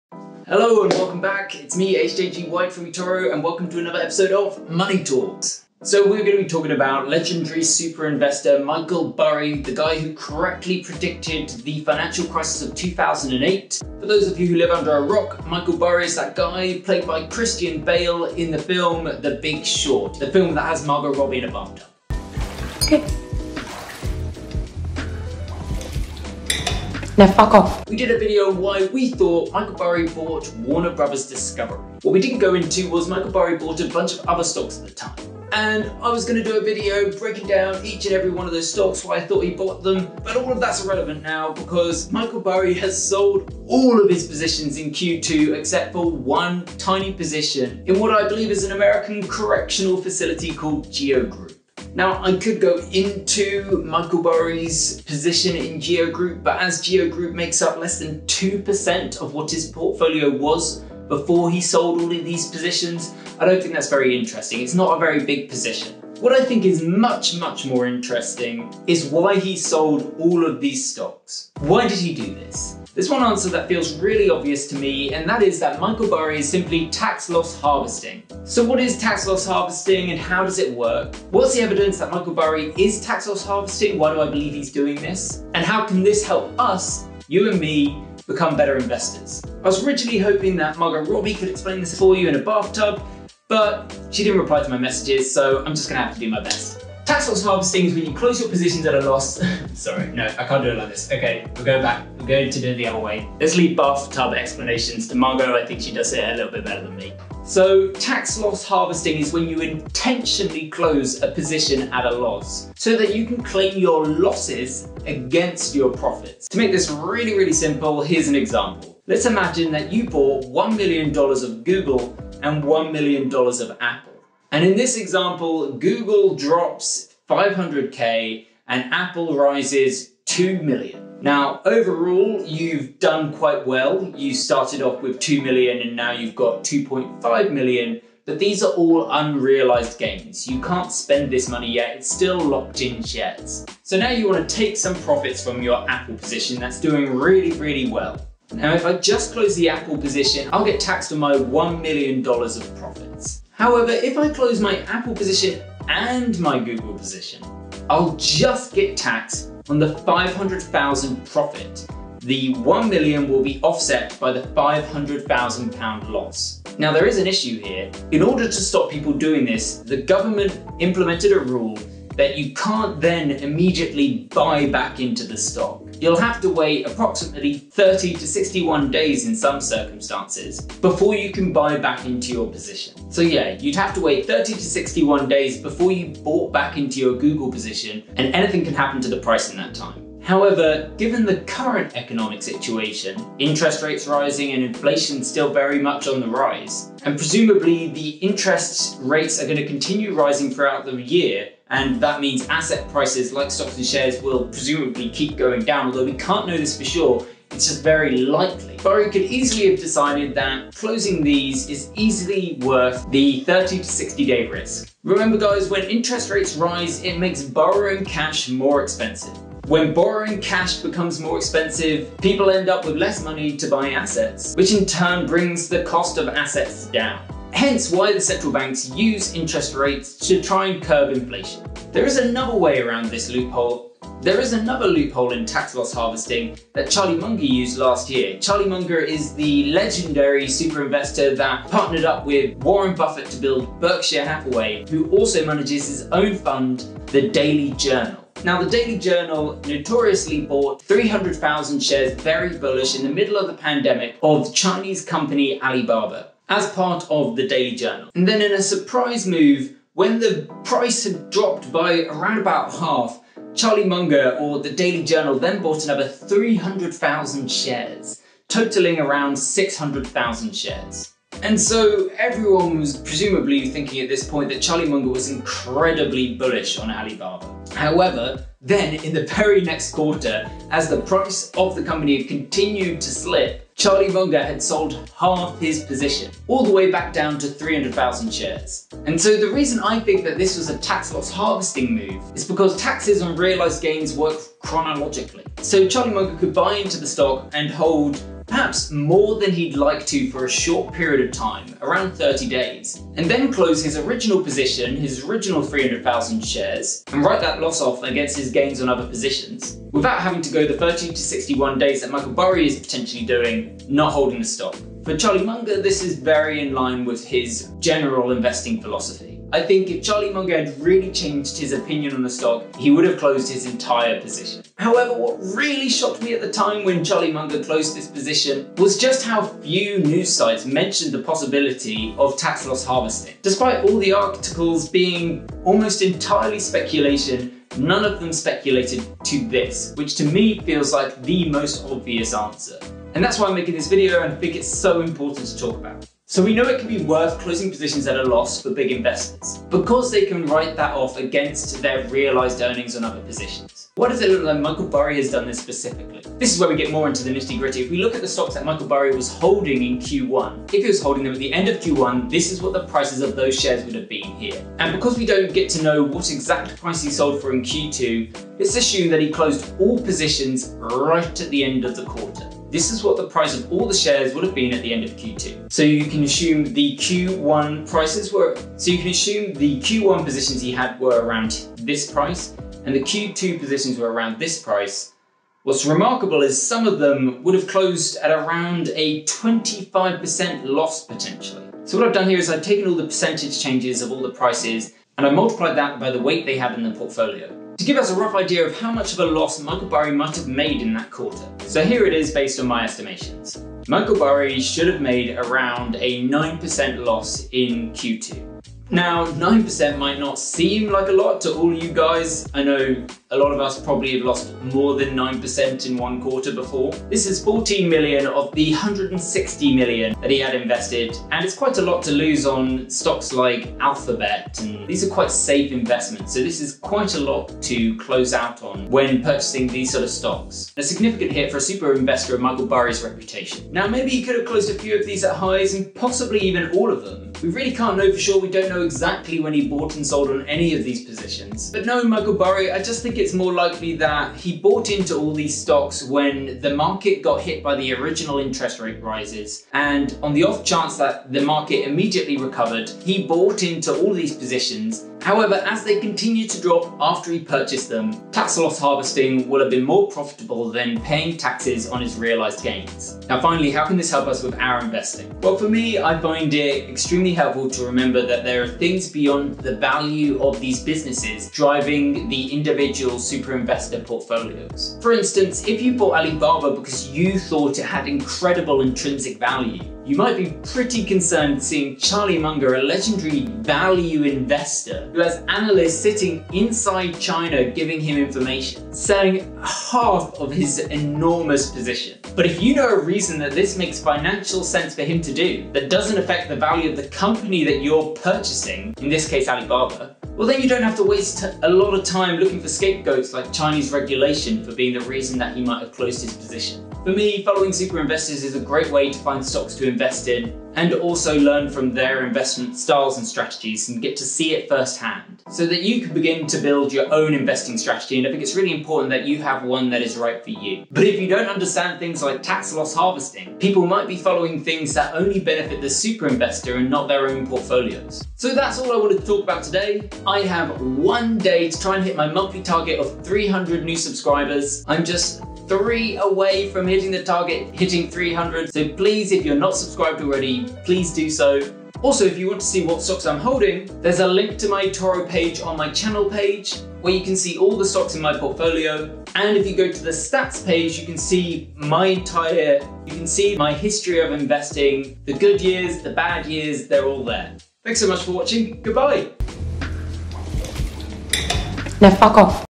I think Michael Burry's made a $14mn Dollar LOSS
Michael Burry (The Big Short investor) has been predicting a big stock market crash and high inflation in 2022. But now his actions have backed up his words. But is this move really as bearish as it seems? Or perhaps he is just Tax Loss Harvesting.

𝗠𝘆 𝗥𝗲𝗰𝗼𝗺𝗺𝗲𝗻𝗱𝗲𝗱 𝗕𝗼𝗼𝗸𝘀 𝗼𝗻 𝗜𝗻𝘃𝗲𝘀𝘁𝗶𝗻𝗴:
1. The Intelligent Investor
2. One up on Wall Street
3. Principles, by Ray Dalio
4. The Psychology of Money
5. Money